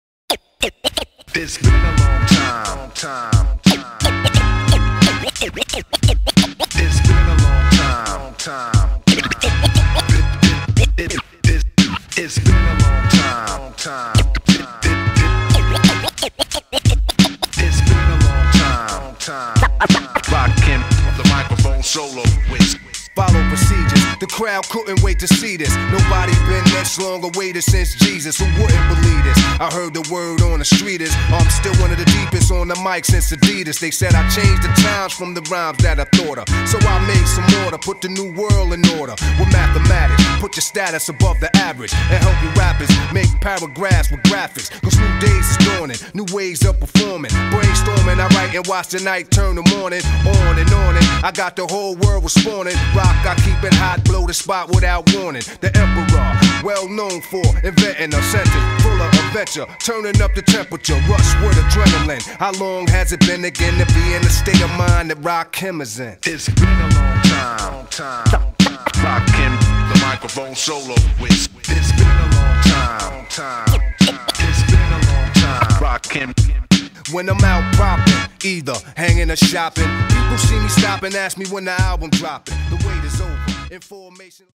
Snapback @ The Federal Bar
"SNAPBACK" @ The Federal Bar in Noho District going down every Thursday night! No cover / no strict dress code / no Top 40, just old school hip-hop & R&B on the 1s n' 2s all night by DJ Ever Ed-e / DJ Stubbs / DJ Bstang representing the Almost Famous DJs + InCrowd crew!